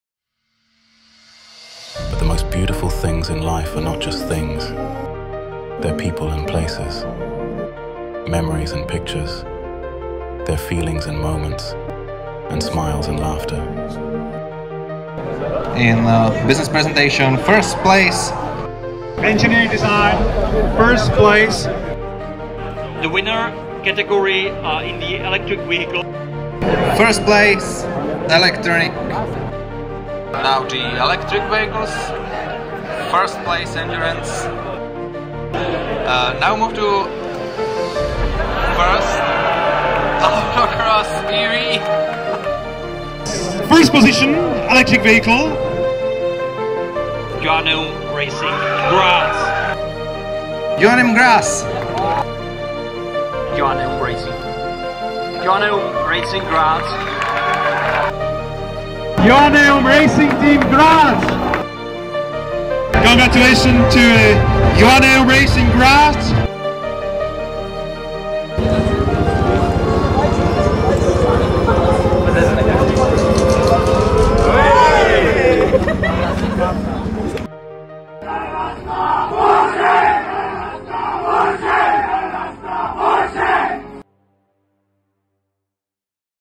FS Czech 2024 - Aftermovie // Joanneum Racing Graz
Overall P1 at Formula Student Czech!🇨🇿❤️
It was an incredible and unbelievable week in the Czech Republic at the Autodrom Most. We got off to a strong start and kept that strength through the whole competition, which led us to a result that still leaves us🤩
🏆Overall Winner
🥇Business Plan Presentation
🥇Engineering Design
🥇Skid Pad
🥇Acceleration
🥇Autocross
🥇Endurance
🥉Efficiency
🏆Garrett Powertrain Innovation Award

Schnitt: Valentina Lohr